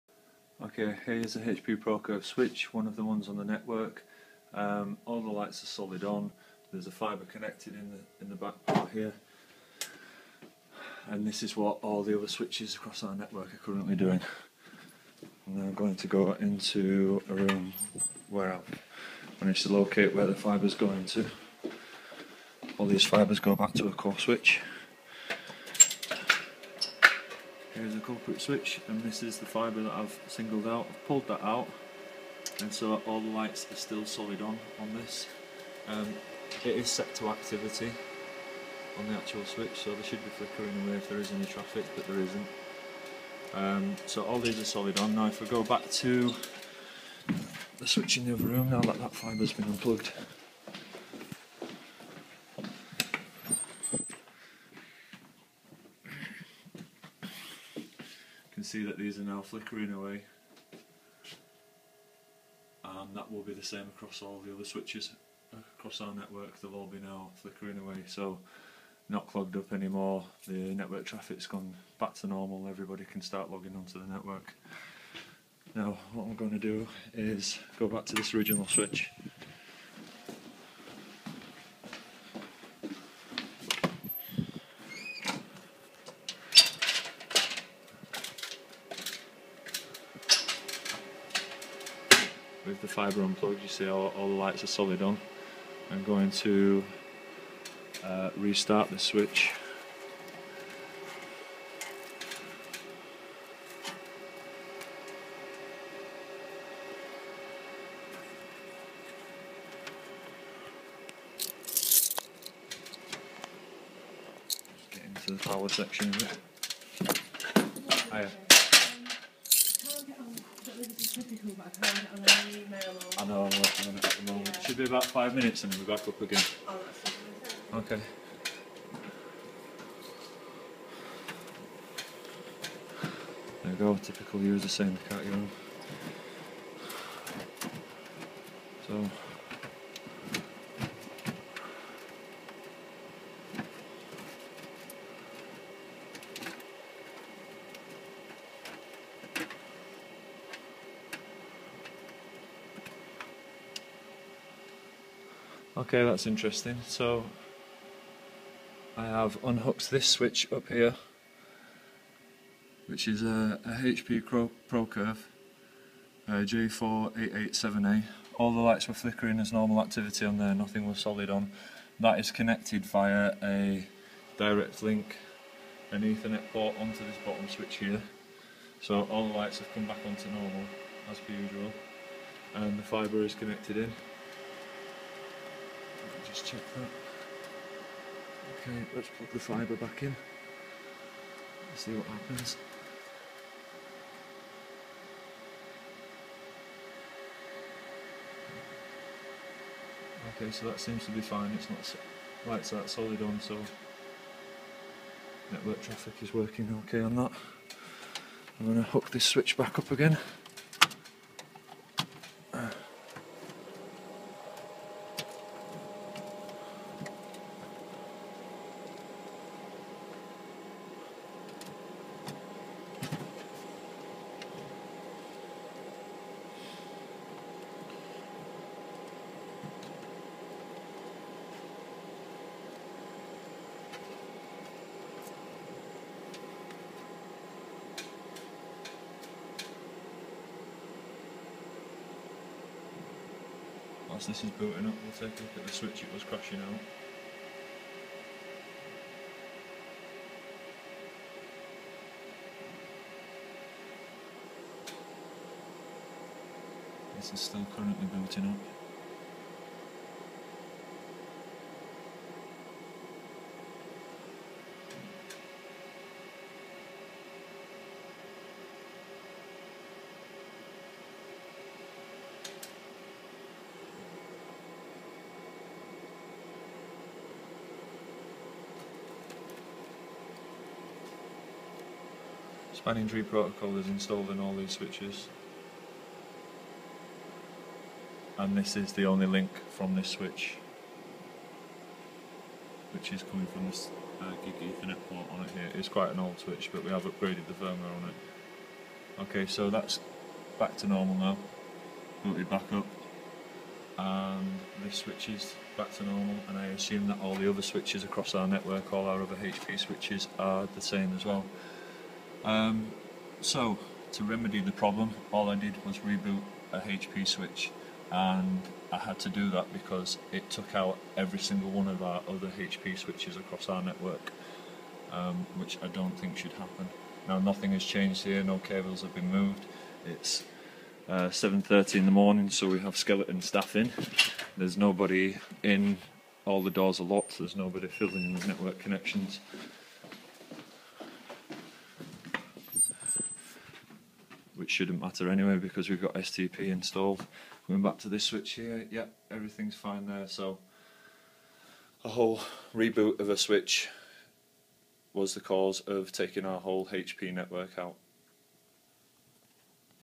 Issue with HP procurve switches crashing out
Result of a faulty fibre cable or network card driver